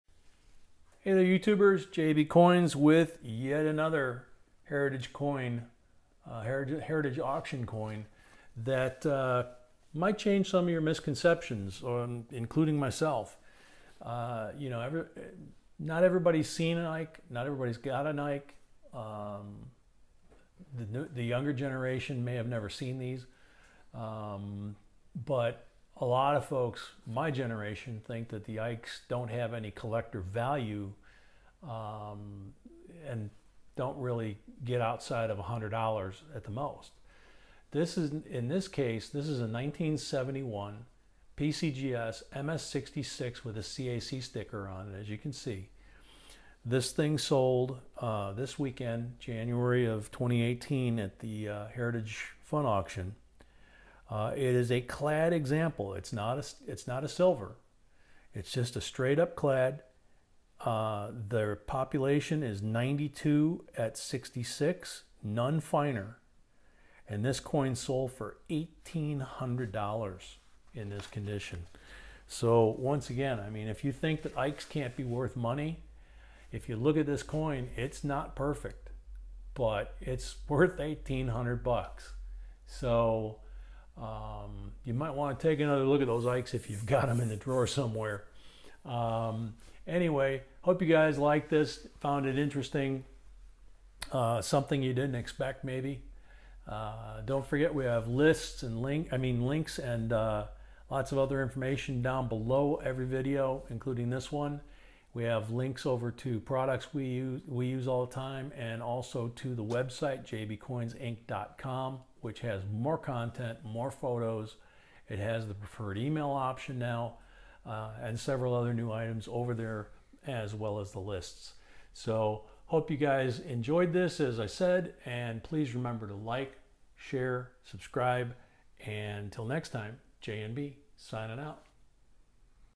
RARE 1971 EISENHOWER DOLLAR WORTH MONEY! RARE IKE DOLLARS TO LOOK FOR!
This beautiful and valuable Eisenhower Dollar coin blows away expectations and makes you re-think about the values of these coins! Dollar coins to look for!!

To check out our coin lists, preferred email support, product links and even more info about coins, go to our